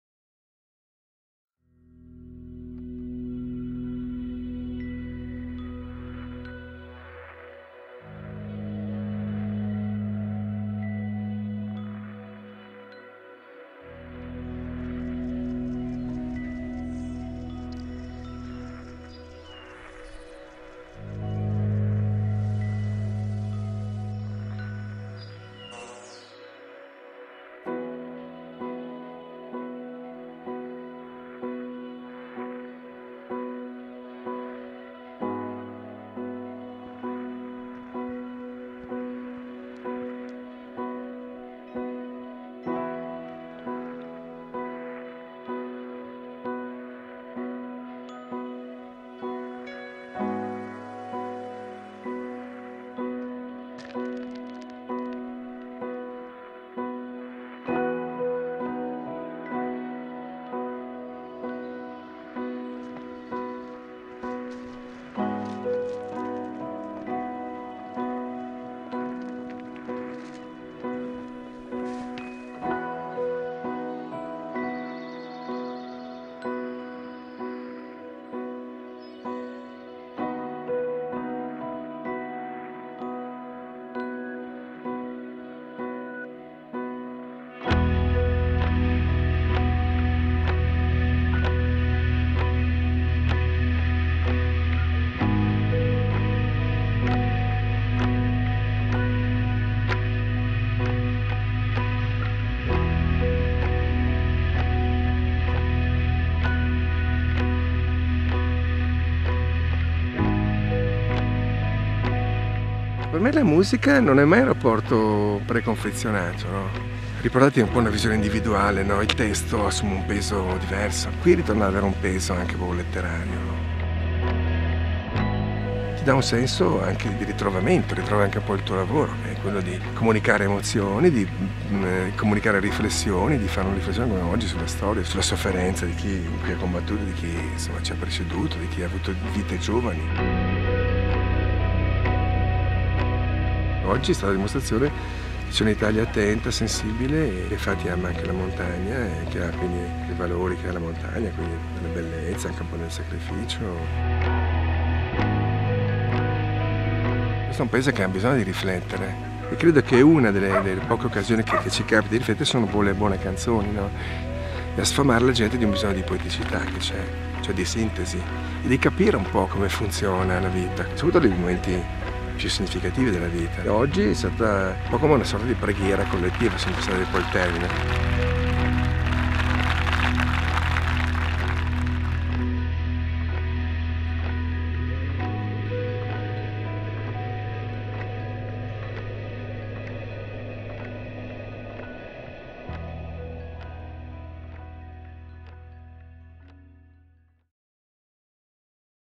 I Suoni delle Dolomiti - Three points of view Ep. 4
Massimo Bubola in Valle dei Laghi, Trentino - 9 agosto 2013
► Vuoi vedere altri video del festival? Clicca qui

"Three points of ...view" è progetto a cura di GiUMa Productions: 3 giovani video-maker raccontano in sei tappe l'esperienza unica del festival I Suoni delle Dolomiti
"Three points of...view" is a project curated by GiUMa Productions: 3 young video makers recount in six stages the unique experience that is the Sounds of the Dolomites festival

Grazie a Matteo Scotton per la realizzazione di questo video/ Thanks Matteo Scotton for making this video